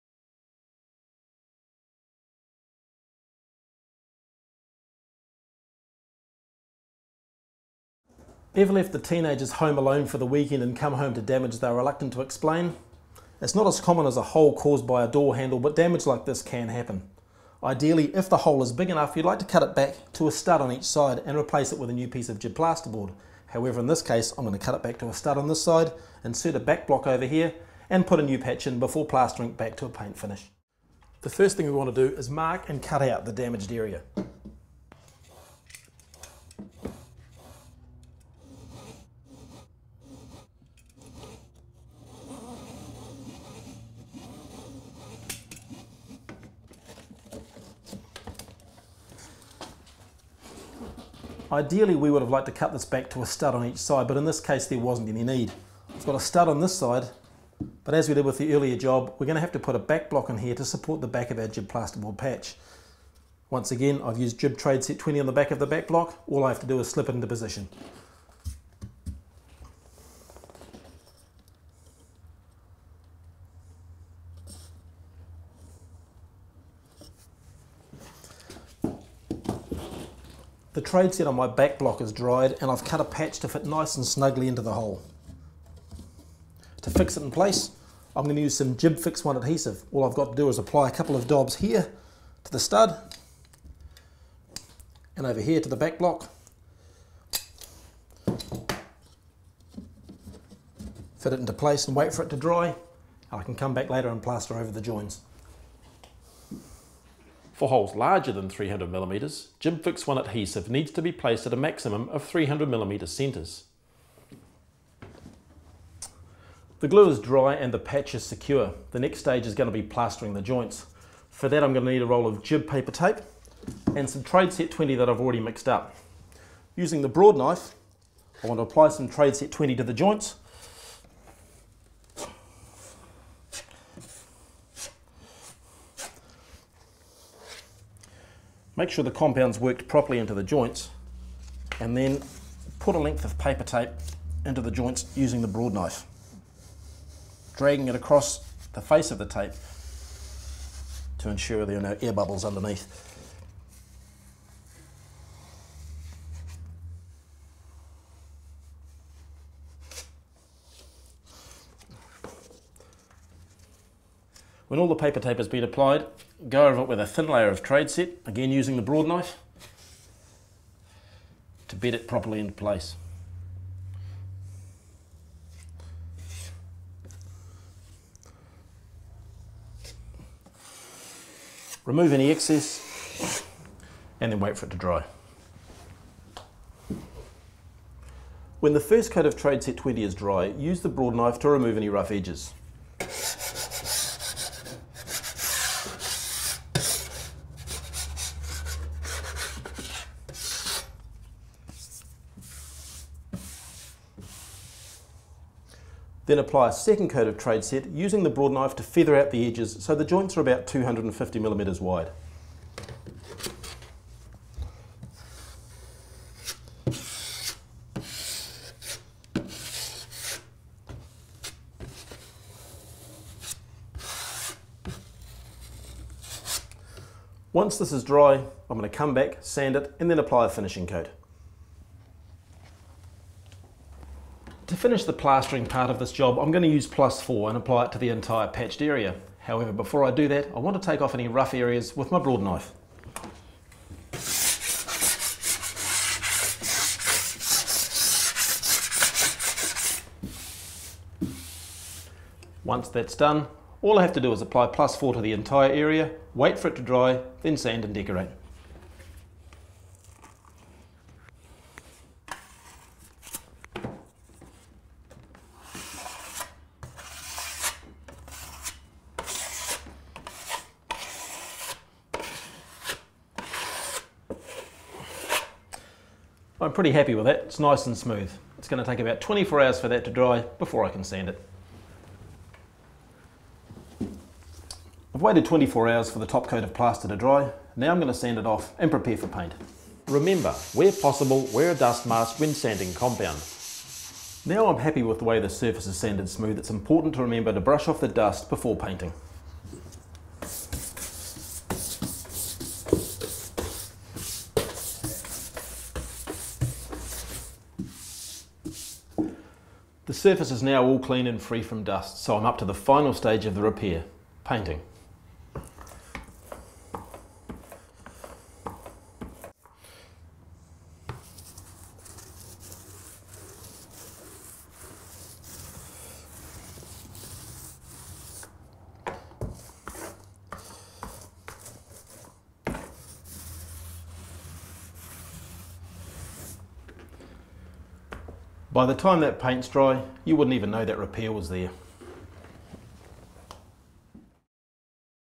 Repairing Large Holes in your plasterboard walls with GIB Living
How to repair larger holes in your plasterboard walls using products such as GIB® Patch Board and GIB Tradeset®.